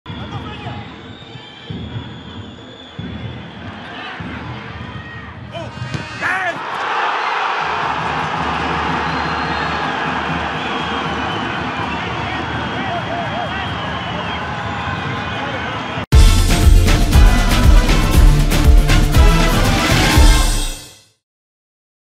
GOL DE PALESTINO 1 vs 0 ESTUDIANTES DE MÉRIDA | CONMEBOL SUDAMERICANA 2023
🇨🇱 Misael Dávila anotó el 1-0 ante Estudiantes de Mérida para el primer tiunfo de los chilenos en la CONMEBOL Sudamericana 2023.